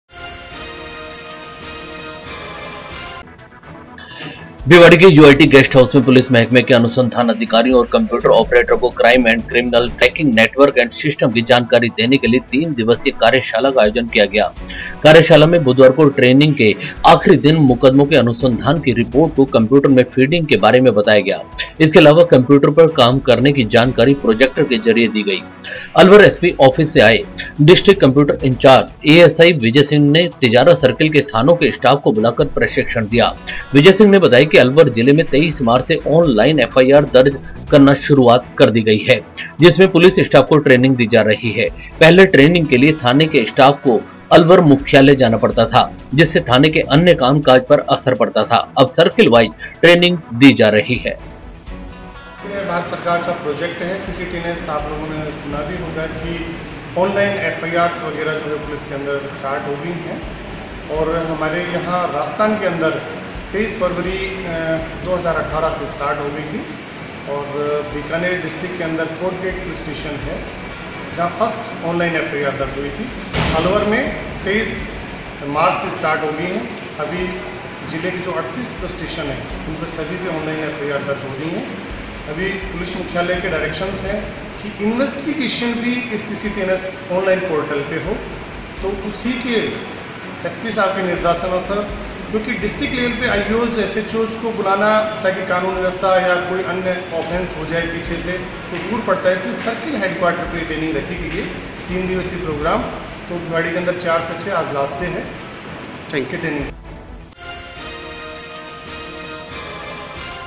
क्राइम एंड क्रिमिनल ट्रैकिंग नेटवर्क सिस्टम की दी जानकारी, पुलिस महकमे के अनुसन्धान अधिकारियों और कं
-भिवाड़ी के युआईटी गेस्ट हॉउस में पुलिस महकमे के अनुसंधान अधिकारियों व कम्प्यूटर ऑपरेटर को क्राईम एण्ड क्रिमिनल ट्रेकिंग  नेटवर्क एण्ड सिस्टम की जानकारी देने के लिए तीन दिवसीय  कार्यशाला का आयोजन किया गया। कार्यशाला में बुधवार को ट्रेनिग के आखिरी दिन मुकदमों के अनुसंधान की रिपोर्ट को कंप्यूटर में फीडिंग के बारे में बताया गया। इसके अलावा कम्प्यूटर पर् काम करने की जानकारी प्रोजेक्टर के ज़रिए दी गई। अलवर एसपी ऑफिस से आये डिस्टिक कम्प्यूटर इंचार्ज एएसआई विजय सिंह ने तिजारा सर्किल के थानो के स्टाफ को बुलाकर प्रशिक्षण दिया। विजय सिंह ने बताया अलवर जिले में 23 मार्च से ऑनलाइन एफ आर आई की शुरुवात हुई है। जिसमे पुलिस स्टाफ को ट्रेनिंग दी जा रही है पहले ट्रेनिंग के लिए थाने के स्टाफ को अलवर मुख्यालय जाना पड़ता था जिससे थाने के अन्य काम काज पर असर पड़ता था। अब सर्किल वाइज ट्रेनिंग दी जा रही है।